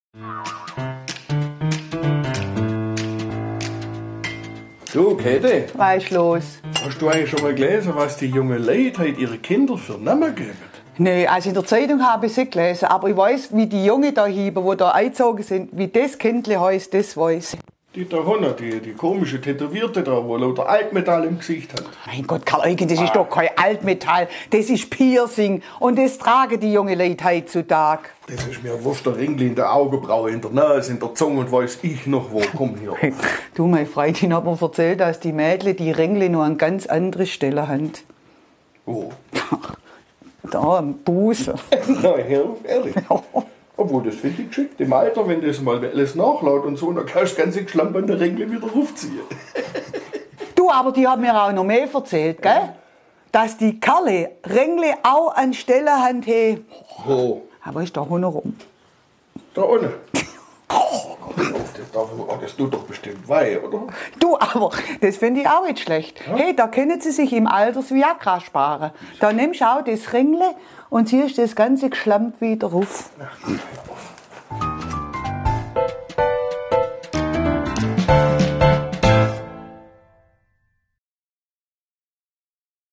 Morgengespräch zur neuen Kultur
Desto irrsinniger, desto beliebter wird jeder Mist angenommen. Wo bleibt die Ethik?